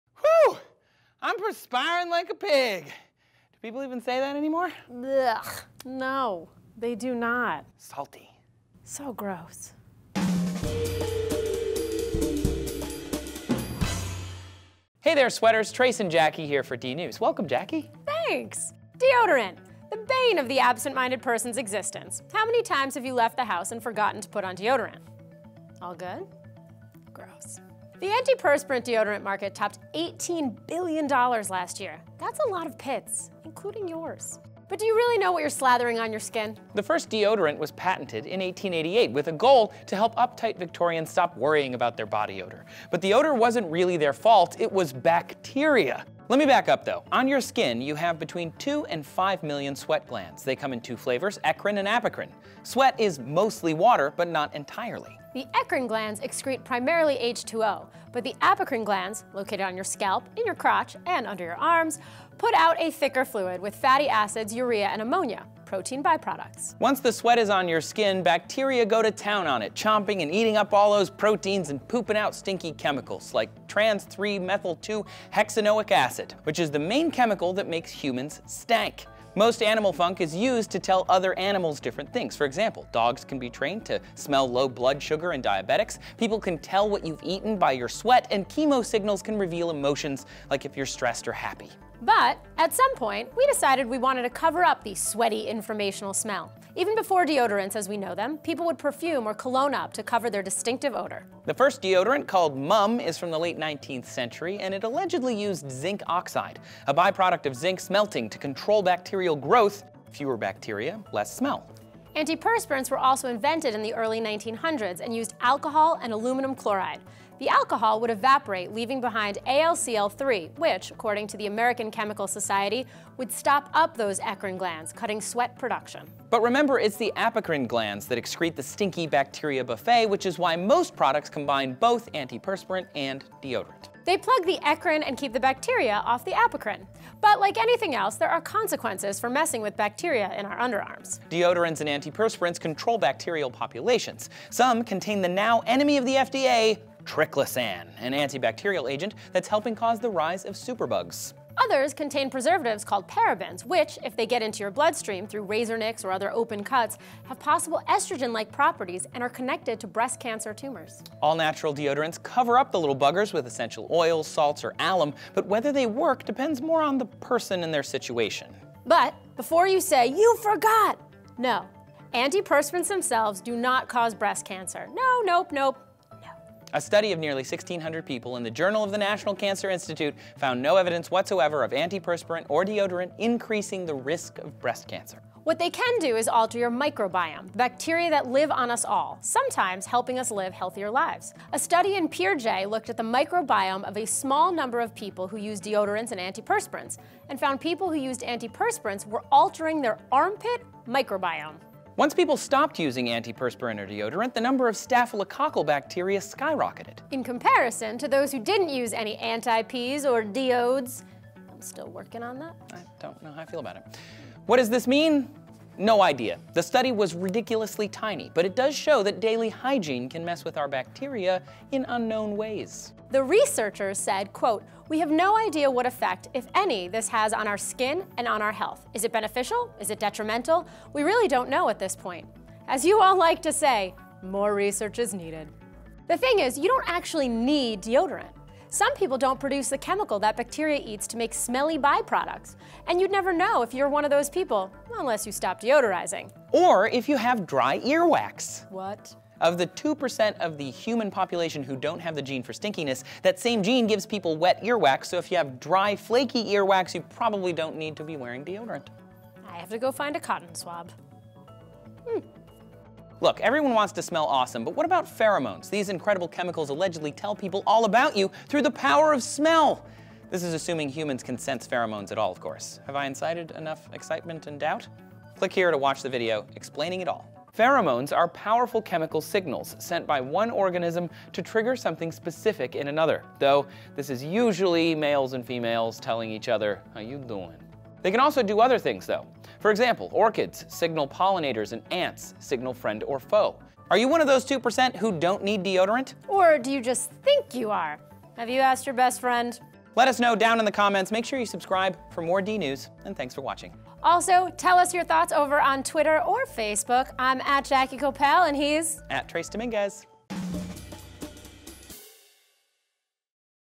What Is Deodorant Really Doing To Your Body?
Most of us use it everyday, but do we know how it works? What does deodorant do to our bodies?

How Advertisers Convinced Americans They Smelled Bad
"In the 1910s deodorants and antiperspirants were relatively new inventions. The first deodorant, which kills odor-producing bacteria, was called Mum and had been trademarked in 1888, while the first antiperspirant, which thwarts both sweat-production and bacterial growth, was called Everdry and launched in 1903."

"The rumor that antiperspirant use causes breast cancer continues to circulate the Internet. Although unfounded, there have been no published epidemiologic studies to support or refute this claim. This population-based case- control study investigated a possible relationship between use of products applied for underarm perspiration and the risk for breast cancer in women aged 20-74 years."

The effect of habitual and experimental antiperspirant  and deodorant product use on the armpit microbiome
"The human armpit has long been noted to host a high biomass bacterial community, and recent studies have highlighted substantial inter-individual variation in armpit bacteria, even relative to variation among individuals for other body habitats. One obvious potential explanation for this variation has to do with the use of personal hygiene products, particularly deodorants and antiperspirants."

Special thanks to Jackie Koppell for co-hosting DNews!